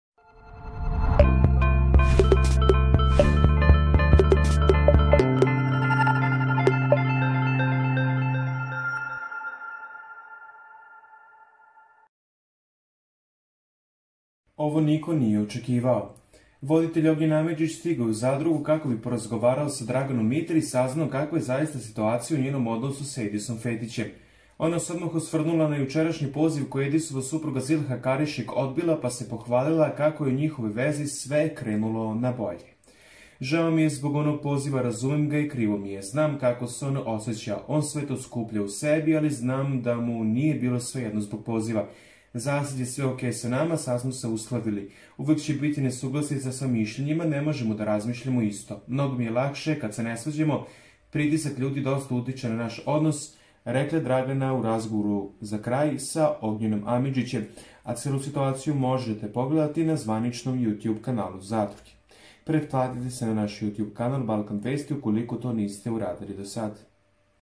Dragana Mitar progovorila o Zilhinom POZIVU, pa otkrila OVO
Mnoge je ovo sokiralo